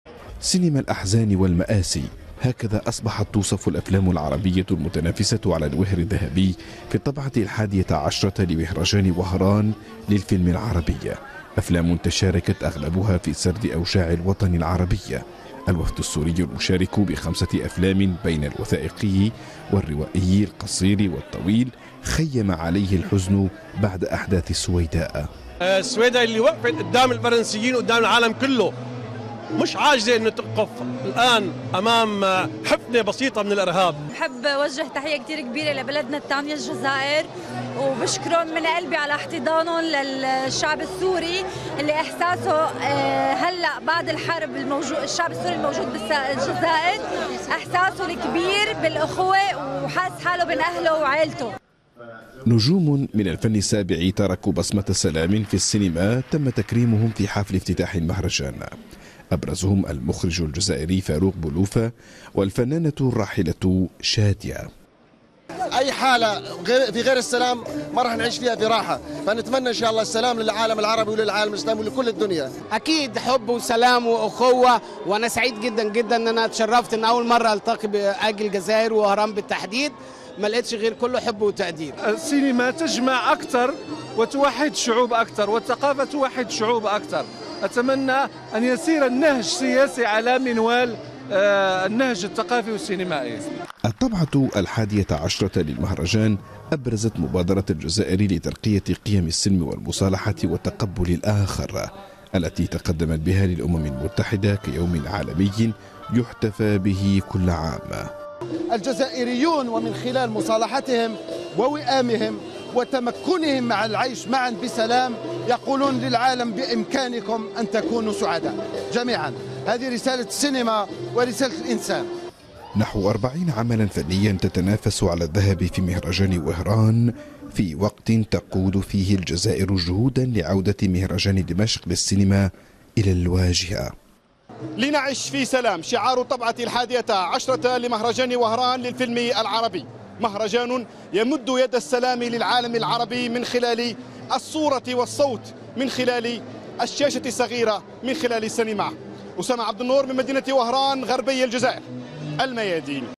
تحت شعار "لنعش معاً في سلام" مهرجان الفيلم العربيّ
إعداد: أسامة عبد النور
تنطلق في مدينة وهران غرب الجزائر النسخة الحادية عشرة لمهرجان الفيلم العربيّ تحت شعار "لنعش معاً في سلام "، ويتنافس في المسابقة التي تخيّم عليها الحزن عقب أحداث السويداء في سوريا نحو 40 فيلماً من 12 دولة عربية.